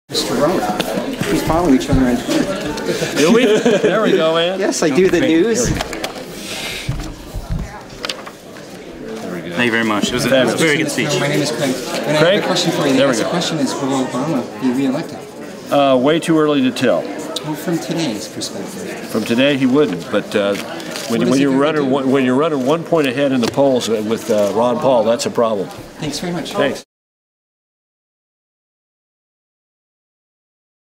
Karl Rove--"Will Obama be reelected?"
At the Karl Rove book signing at the U of M in Minneapolis, I asked Karl Rove if he thought Obama would be reelected while he signed my book. I have more of the meeting, but I'm also uploading this question alone. Mr Rove at first says, "It's way too early", but I press him, "from today's perspective", and then he offers that Obama looking from today polls just 1% better than Ron Paul, and he doesn't look like he'll be reelected.